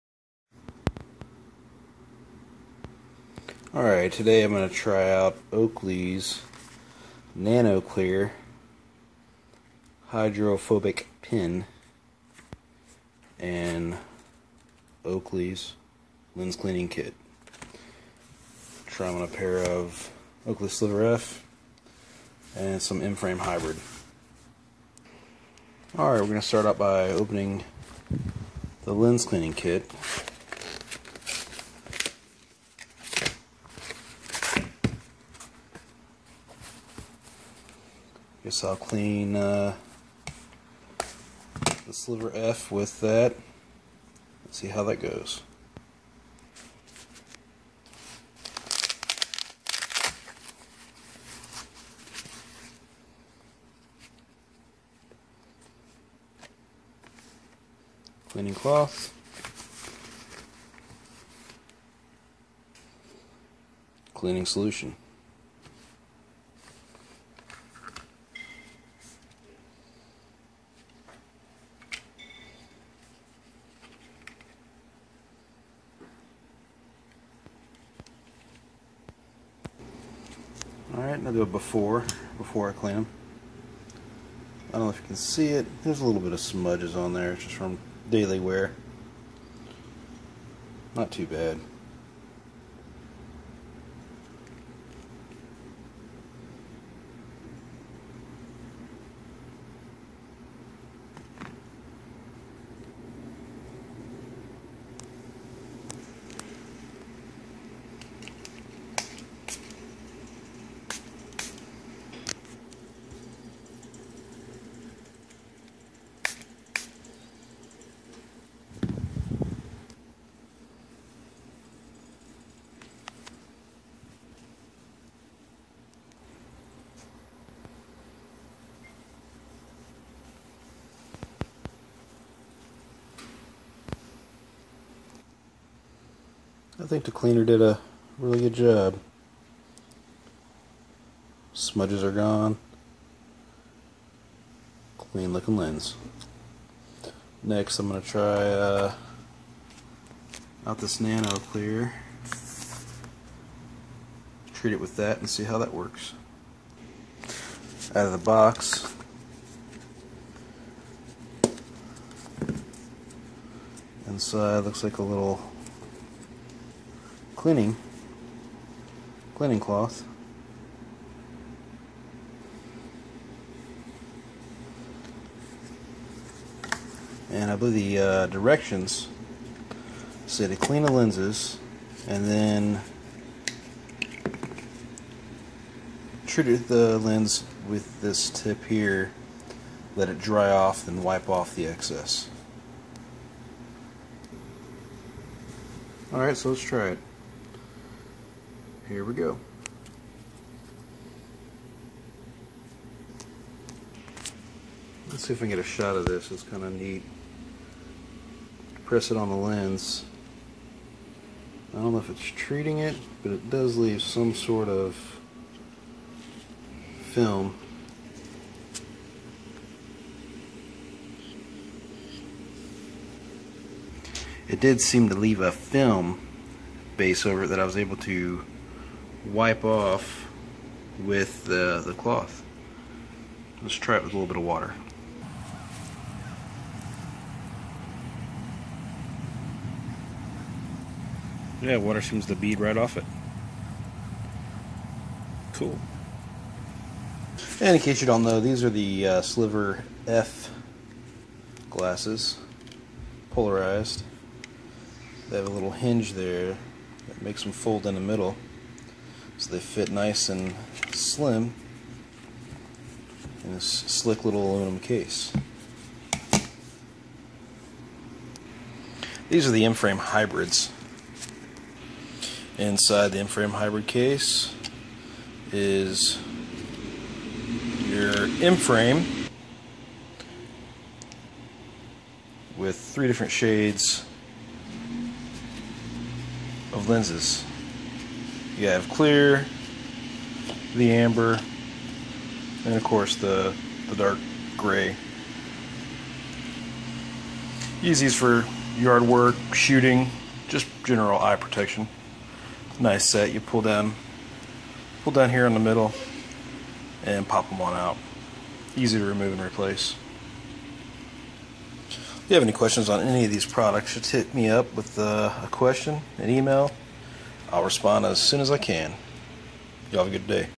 Cleaning the Oakley Sliver F with Nanoclear Hydrophobic.
Oakley's Nanoclear Hydrophobic witg their Lens Cleaning kit.   Sliver F and M-Frame Hybrid.  Stock No. 20-S1242-B